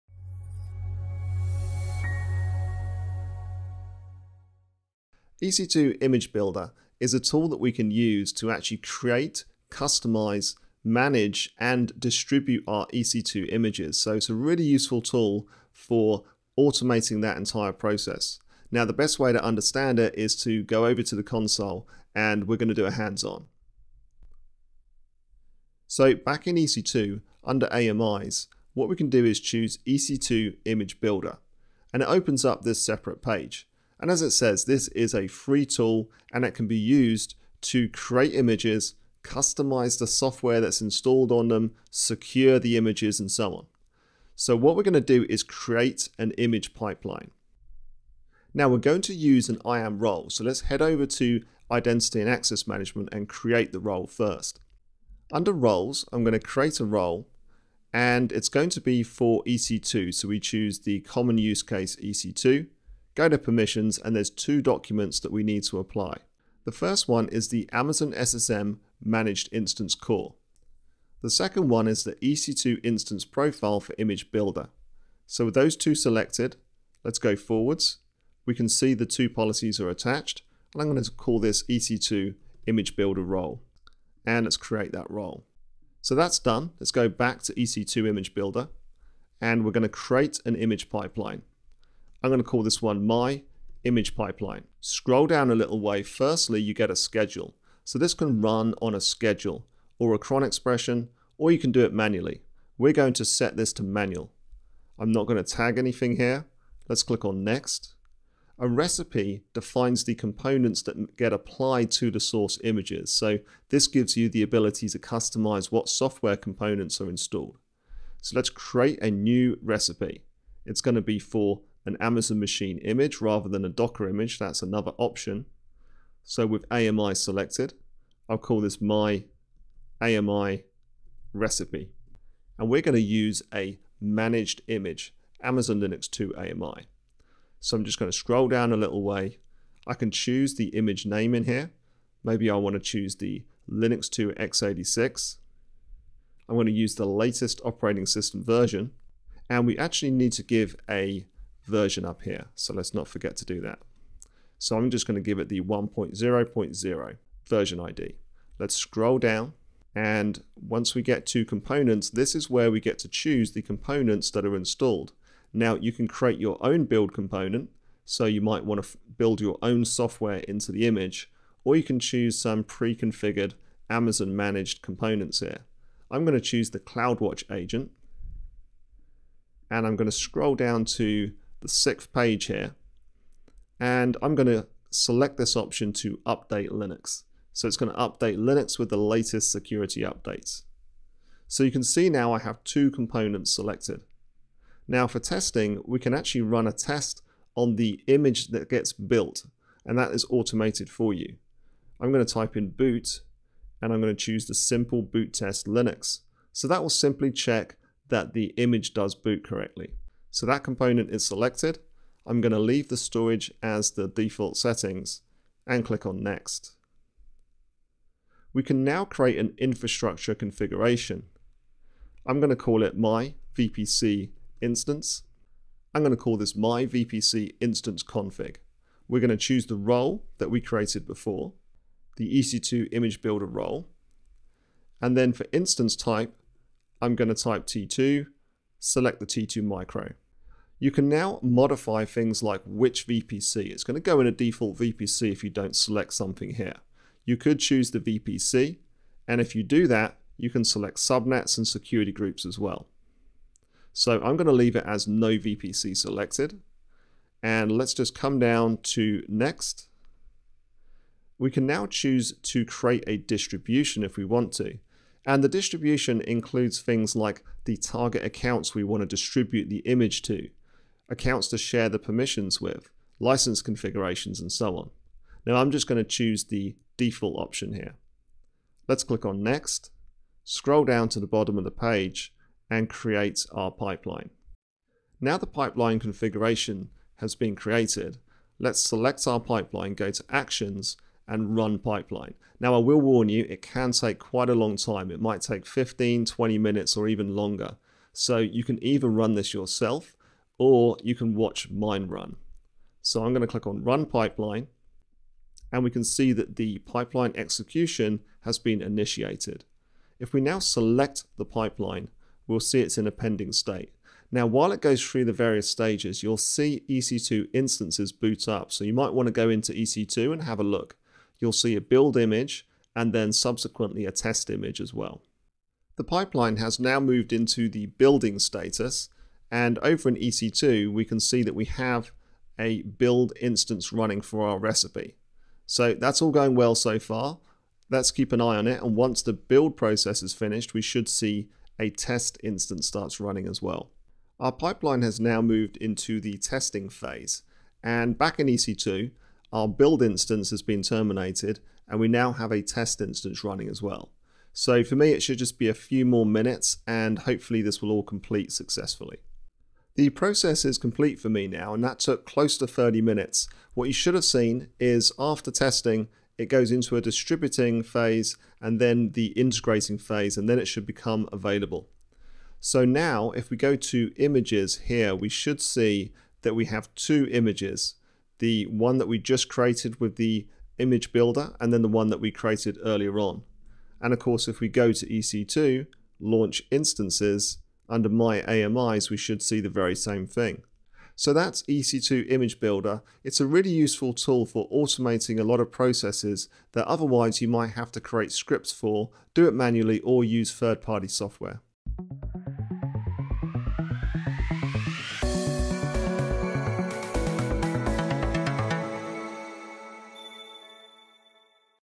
EC2 Image Builder
EC2 Image Builder can be used to create and manage images for virtual machines and containers. You can build, test, and deploy images to AWS resources and on-premises servers.

📌 Timestamps
0:00 - Introduction
0:24 - IAM Role Setup
1:26 - Pipeline Creation
2:37 - Component Selection
3:37 - Infrastructure Configuration
4:38 - Pipeline Execution
5:03 - Build Phase Observation
6:12 - Completion & Verification

In this AWS video tutorial, you'll see how to use EC2 Image Builder to create an image and deploy and test it through a pipeline.

This video comes from our new AWS Certified Solutions Architect Associate course. If you're preparing for your AWS Certification exam, get access to the full course using the